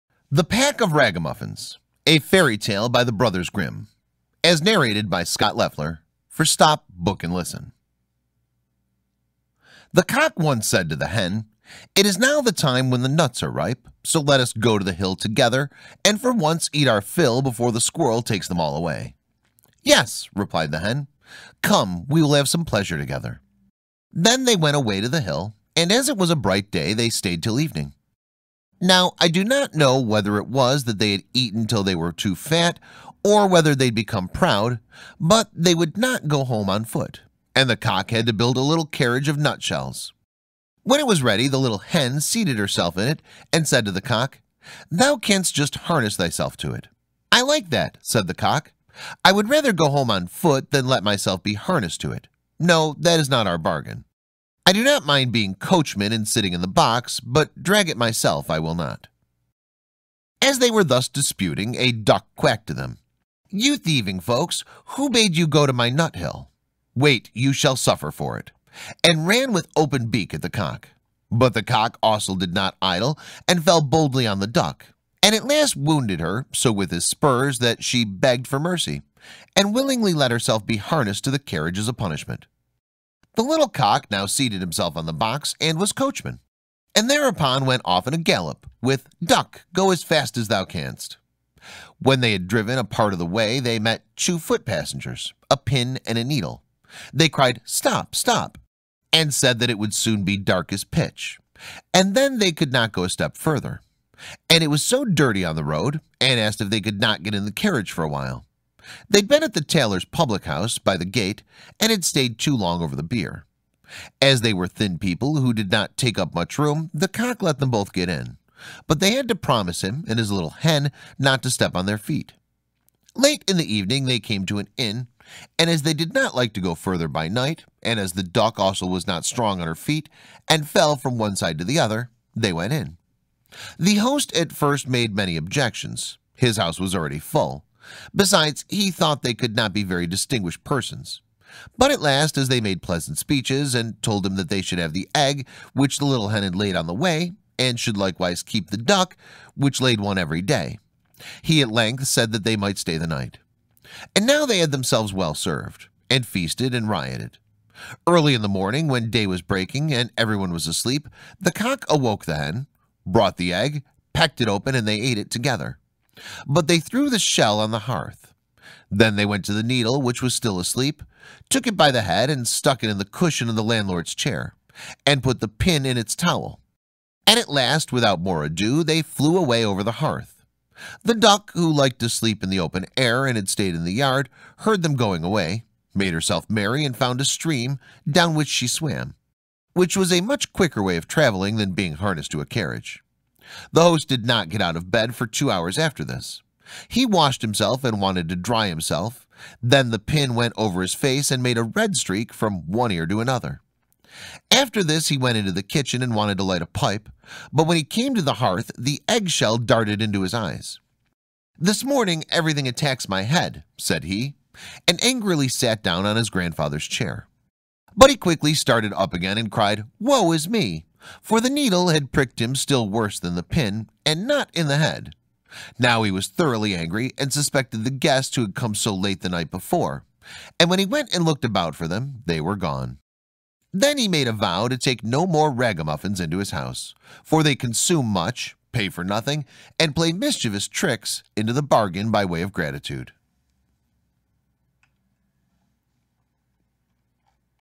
Brothers Grimm -- 010 The Pack of Ragamuffins
A cock and a hen are lazy and up to no good. A Grimm's Fairytale.

Narrated by Scott Leffler.

For Stop Book & Listen.